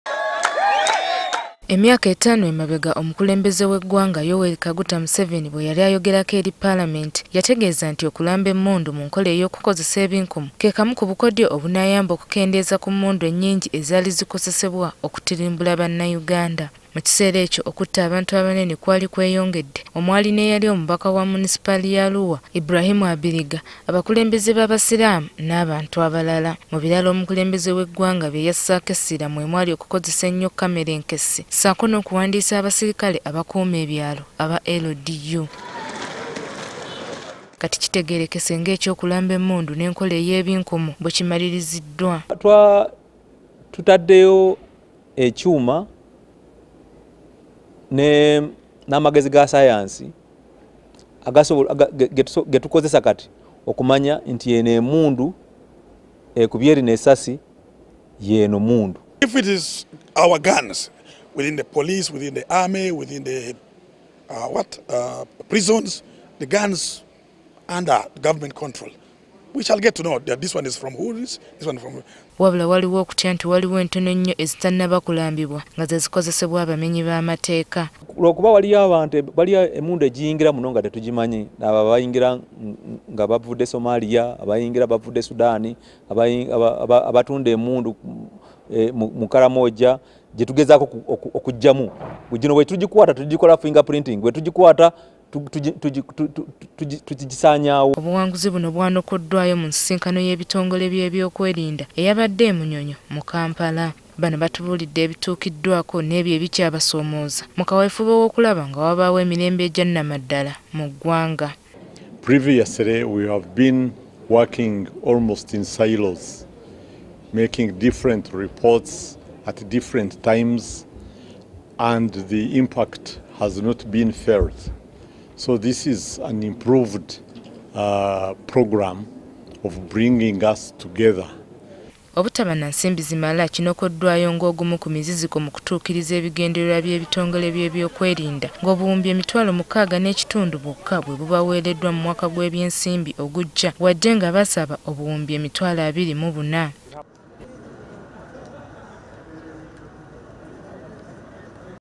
OKULAMBA EMMUNDU :Ab’ebyokwerinda baliko webatuuse
Ebitongole byeby'okwerinda bimativvu nti kati birina obusobozi obumala  okumanya emmundu eziba zikozeseddwa ba kyala kimpadde  bweziba nga zirina akakwate neza Gavumenti, ez'ebitongole ebyobwannanyini oba abantu sekinoomu kasita baba nga bazifuuna mu makubo matuufu okuva mu bebyokwerinda.Kino kyekimu ku bituukiddwako  nga abebyokwerinda besigama ku nkola ey'okukozesa ebinkumu okulamba emmundu zino .

Amyuka omuduumizi w'amagye  agamba nti kino kikoleddwa  ku mmundu ebitundu 99 ku kikumi .